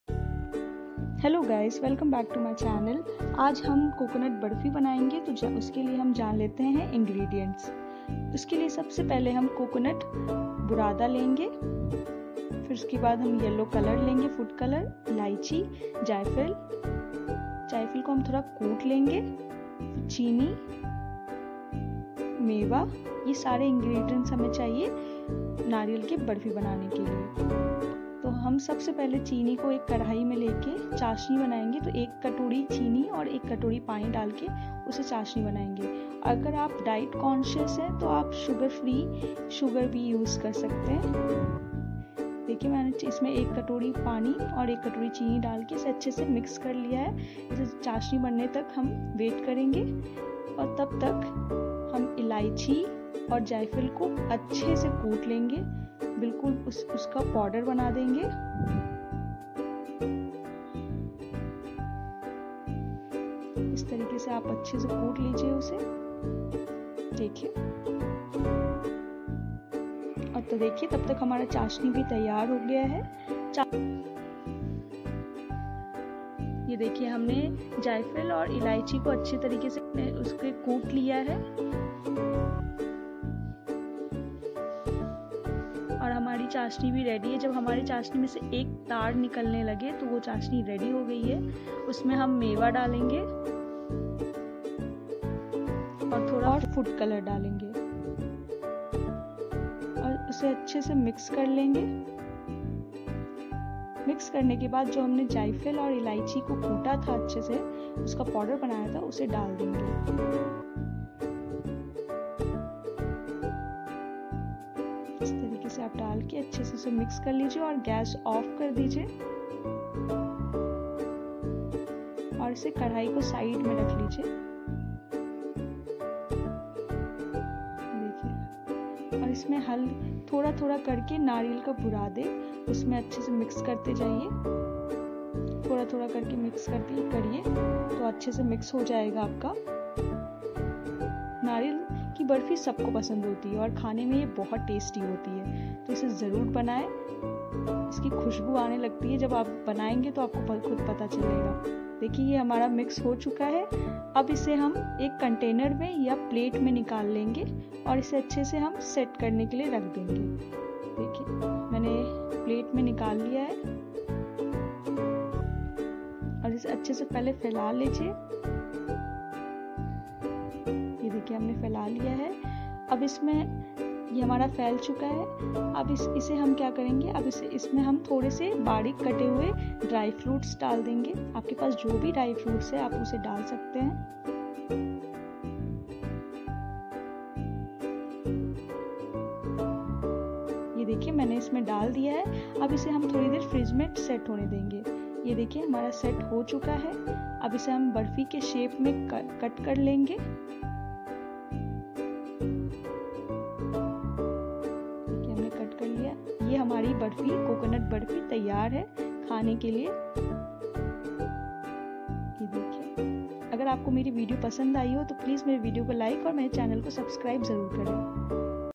15 min mai banaye bazar jesa COCONUT BARFI || Kam samano se banaye tasty COCONUT BARFI
15 min mai banaye bazar jesa COCONUT BARFI || Kam samano se banaye tasty COCONUT BARFI

Pre Time :- 5 min
Cook Time :- 15 min
Number of servings :- 5-7 person

Ingredients:-
1) Coconut Powder 250g
2) Sugar 1 Small Bowl
3) Cardomon 2-3
4) Yellow Food Colour
5) Nutmeg (Jayfal) 1
6) Meva (optional)
7) For Garnishing- Chopped Dry Fruits

Instagram I'd:-